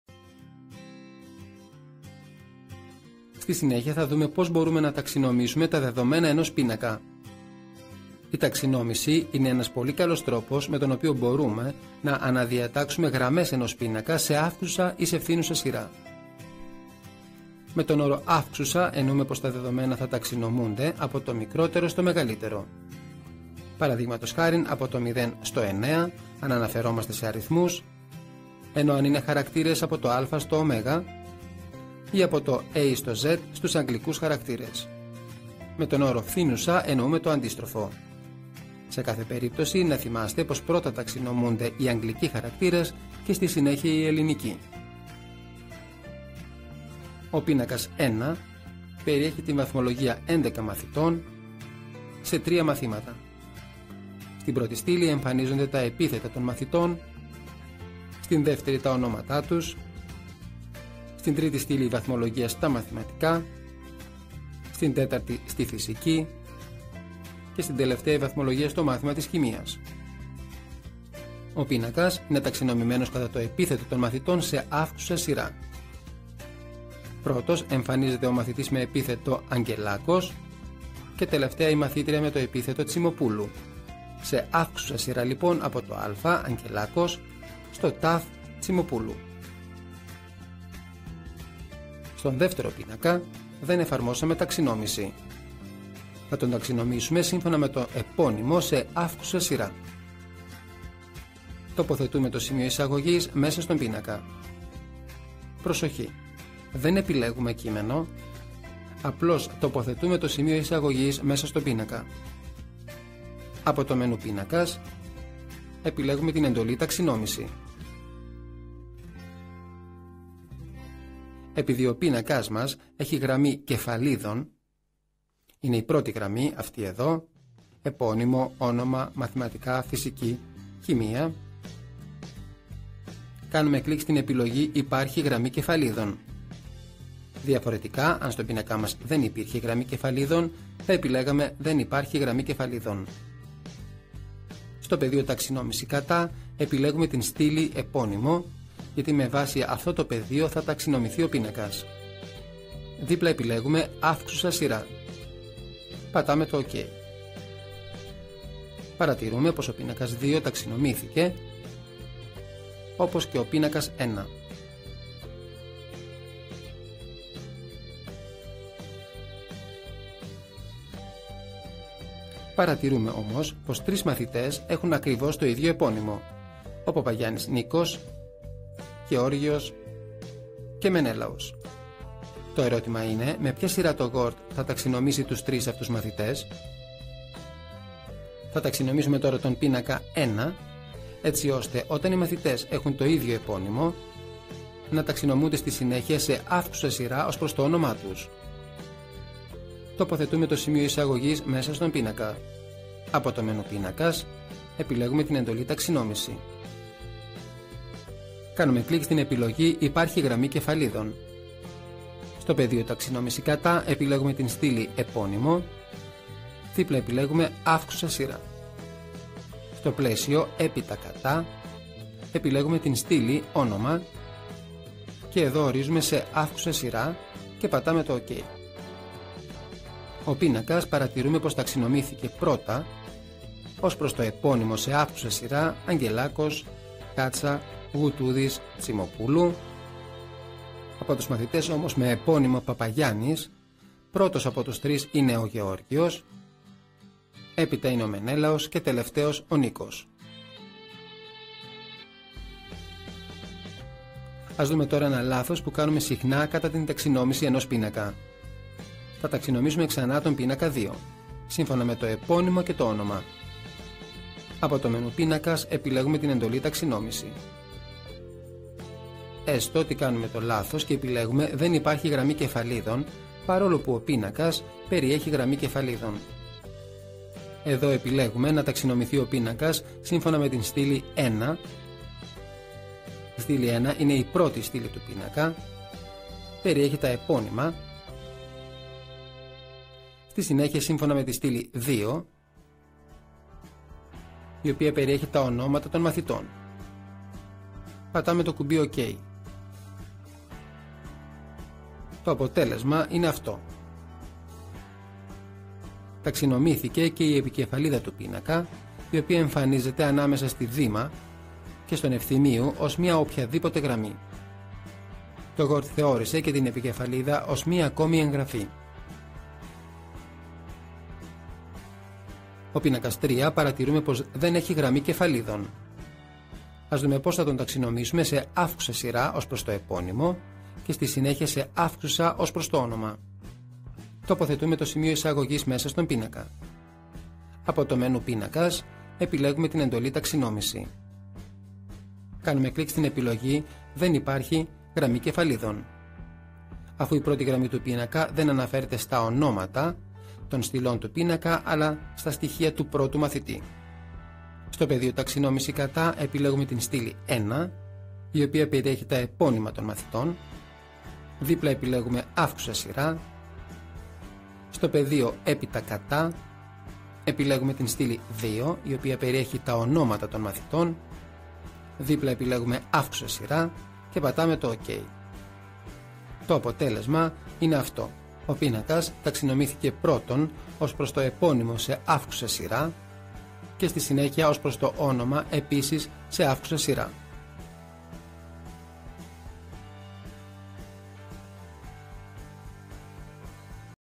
Word 2003 Expert - Ταξινόμηση πίνακα
Στη συνέχεια θα δούμε πως μπορούμε να ταξινομήσουμε τα δεδομένα ενός πίνακα. Η ταξινόμηση είναι ένας πολύ καλός τρόπος με τον οποίο μπορούμε να αναδιατάξουμε γραμμές ενός πίνακα σε αύξουσα ή σε φθίνουσα σειρά. Με τον όρο αύξουσα εννοούμε πως τα δεδομένα θα ταξινομούνται από το μικρότερο στο μεγαλύτερο. Π.χ. από το 0 στο 9, αν αναφερόμαστε σε αριθμούς, ενώ εάν είναι χαρακτήρες από το α στο ω, ή από το a στο z στους αγγλικούς χαρακτήρες. Με τον όρο φθίνουσα εννοούμε το αντίστροφο. Σε κάθε περίπτωση να θυμάστε πως πρώτα ταξινομούνται οι αγγλικοί χαρακτήρες και στη συνέχεια οι ελληνικοί. Ο «πίνακας1», περιέχει την βαθμολογία έντεκα μαθητών, σε τρία μαθήματα. Στην πρώτη στήλη εμφανίζονται τα επίθετα των μαθητών, στην δεύτερη τα ονόματα τους, στη τρίτη στήλη η βαθμολογία στα Μαθηματικά, στην τέταρτη η βαθμολογία στην Φυσική, και στην τελευταία η βαθμολογία στο μάθημα της Χημείας. Ο πίνακας είναι ταξινομημένος κατά το επίθετο των μαθητών, σε αύξουσα σειρά. Πρώτος εμφανίζεται ο μαθητής με το επίθετο ο «Αγγελάκος», και τελευταία η μαθήτρια με το επίθετο «Τσιμοπούλου». Σε αύξουσα σειρά λοιπόν από το «α» (Αγγελάκος) στο «τ» (Τσιμοπούλου). Στον δεύτερο πίνακα, δεν εφαρμόσαμε ταξινόμηση. Θα τον ταξινομήσουμε σύμφωνα με το επώνυμο σε αύξουσα σειρά. Τοποθετούμε το σημείο εισαγωγής μέσα στον πίνακα. Προσοχή, δεν επιλέγουμε κείμενο, απλώς τοποθετούμε το σημείο εισαγωγής μέσα στον Πίνακα. Από το μενού «Πίνακας», επιλέγουμε την εντολή «Ταξινόμηση». Επειδή ο πίνακας μας έχει γραμμή κεφαλίδων, είναι η πρώτη γραμμή, αυτή εδώ, «Επώνυμο», «Όνομα», «Μαθηματικά», «Φυσική» και «Χημεία», κάνουμε κλικ στην επιλογή «Υπάρχει γραμμή κεφαλίδων». Διαφορετικά εάν στον πίνακα μας δεν υπήρχε η γραμμή κεφαλίδων, θα επιλέγαμε «Δεν υπάρχει γραμμή κεφαλίδων». Στο πεδίο «Ταξινόμηση κατά» επιλέγουμε τη στήλη «Επώνυμο», γιατί με βάση αυτό το πεδίο θα ταξινομηθεί ο πίνακας. Δίπλα επιλέγουμε «Αύξουσα» σειρά. Πατάμε το κουμπί ΟΚ. Παρατηρούμε πως ο «πίνακας2» ταξινομήθηκε, όπως και ο «πίνακας1». Παρατηρούμε όμως, πως τρεις μαθητές έχουν ακριβώς το ίδιο...